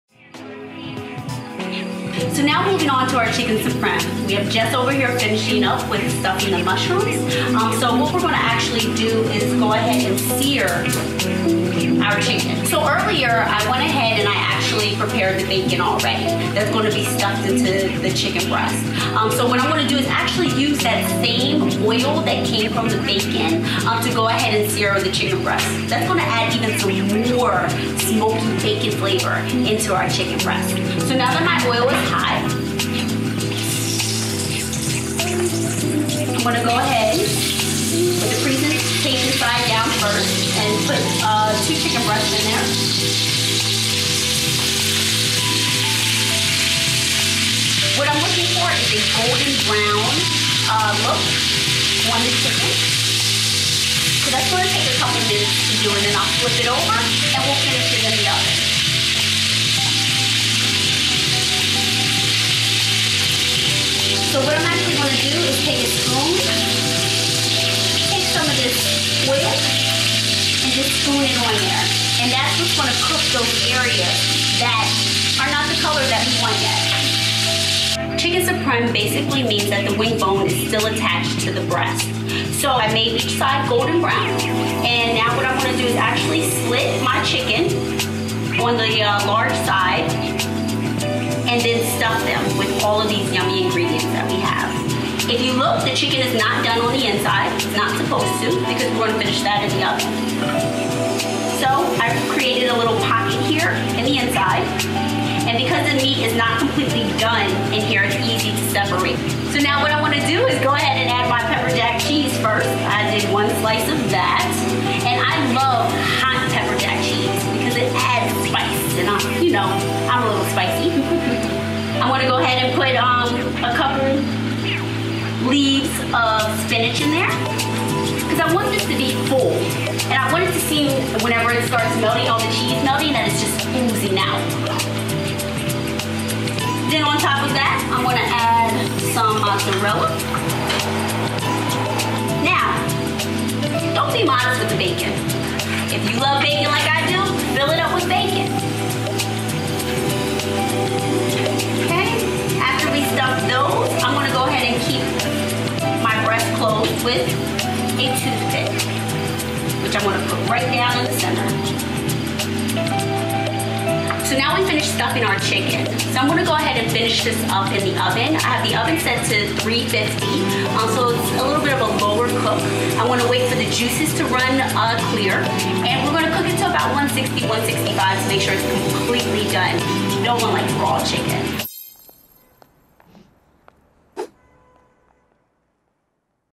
Stuffed Chicken Supreme
Chef Nickie gives the low down on how to make a succulent stuffed chicken signature dish.